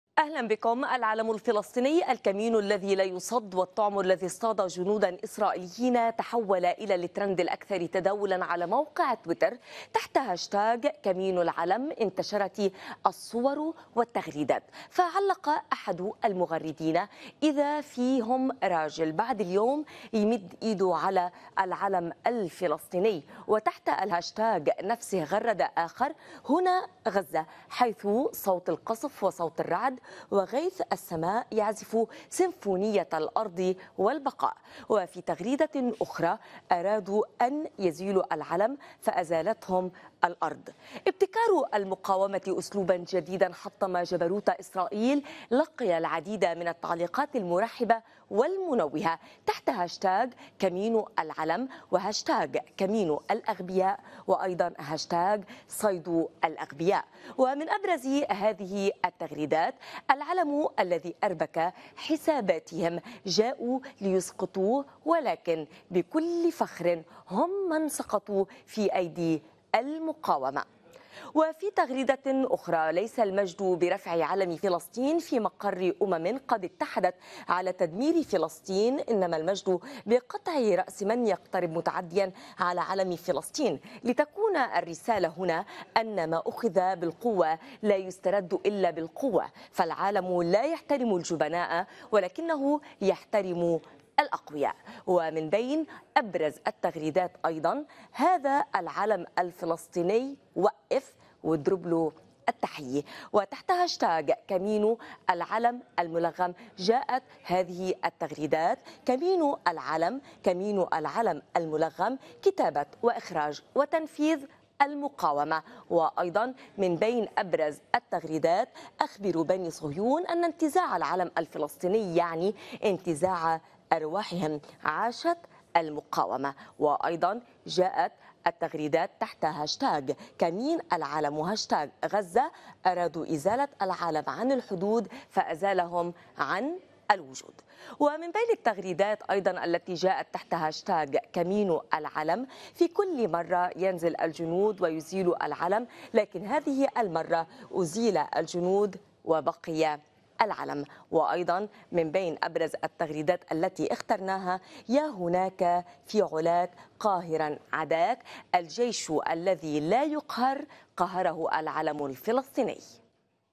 هاشتاغ "كمين العلَم" يتصدّر تويتر
إعداد: بيسان طراف
في ضوء العملية الفلسطينية النوعية تصدّر هاشتاغ "كمين العلَم" تويتر  فانتشرت التغريدات والصور والتعليقات المنوّهة بهذه العملية.